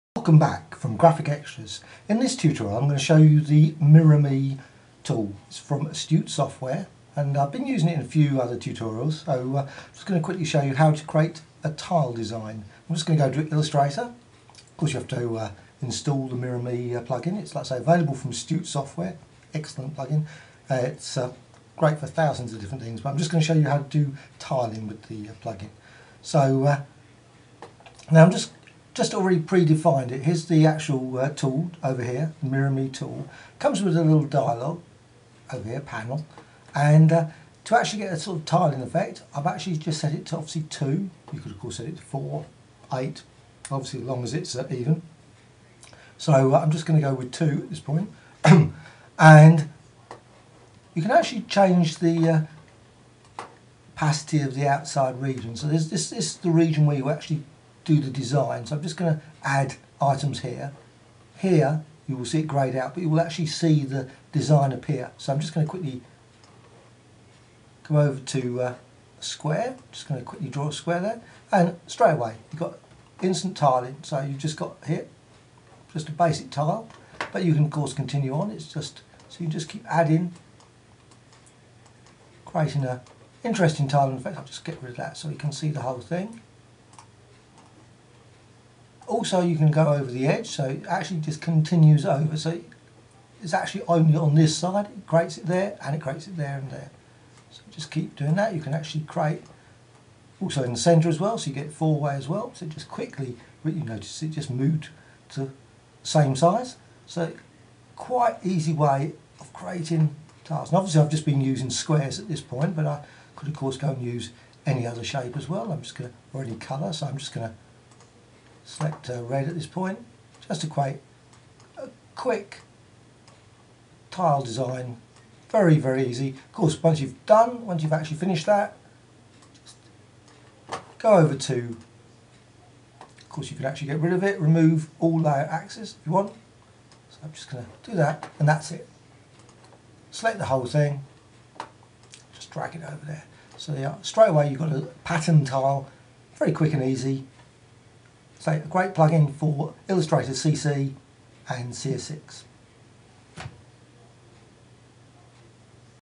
MirrorMe Plug-in Tiling Effect In Illustrator | How To Tutorial | Graphicxtras
A  tutorial showing the mirrorme plugin from astute graphics and the creation of tiling in Illustrator CC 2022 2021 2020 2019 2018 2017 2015 (CS6 CS5)